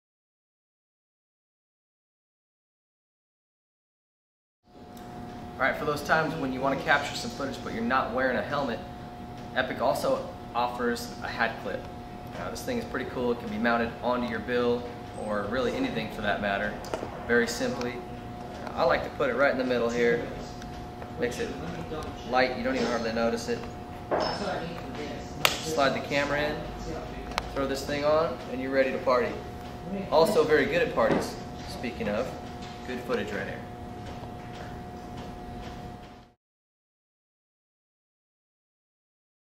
How To - Attach a hat clip
Watch as TLD's David Pingree explains how to use the Epic Hat Clip mount.